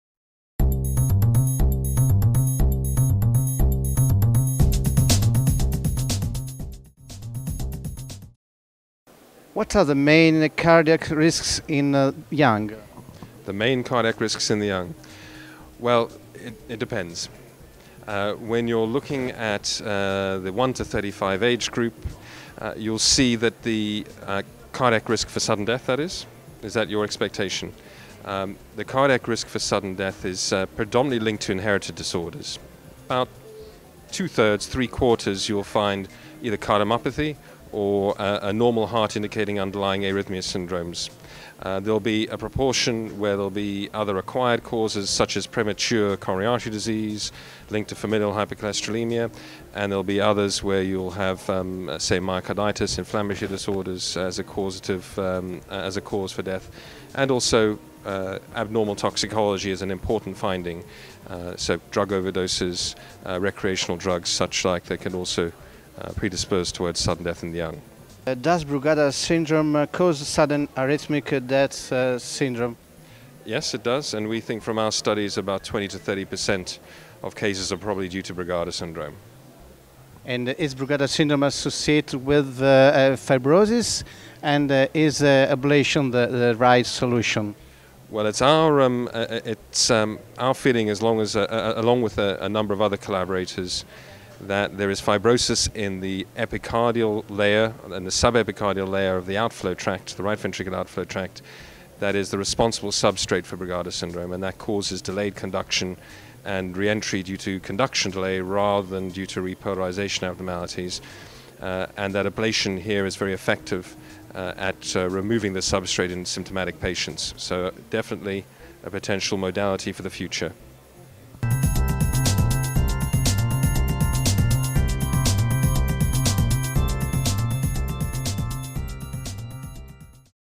Elijah BEHR: "ARVC and Brugada syndrome: what is new? "
Elijah BEHR - London (UK)
ARVC and Brugada syndrome: what is new?

Congress
THE RIGHT HEART. THE NEW FRONTIER“
Padua (Italy), March 1st-3rd, 2017